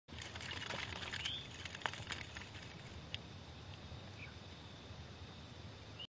Wherever There's Water, There’s Climbing Perch!
The climbing perch, a species of amphibious freshwater fish, is native to Cambodia. You'll find these in any body of water.